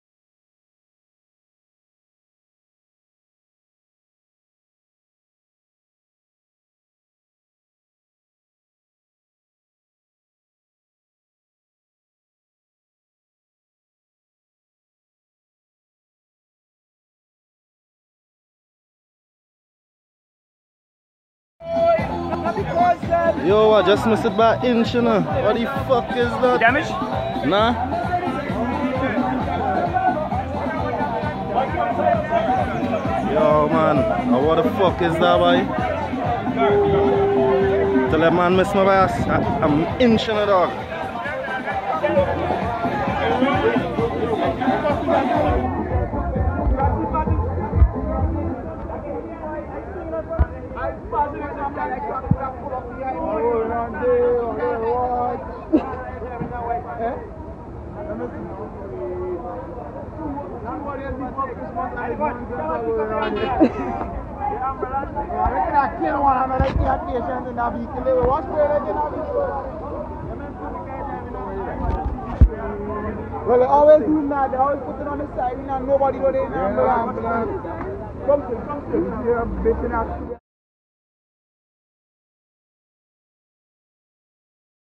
AMBULANCE GONE WILD | Ambulance goes airborne in River Road, WATCH what happens next
If you have any videos of bad driving/drivers in Grenada, Carriacou and Petite Martinique, feel free to send them to us at . Video will be credited to you except you wish to remain anonymous.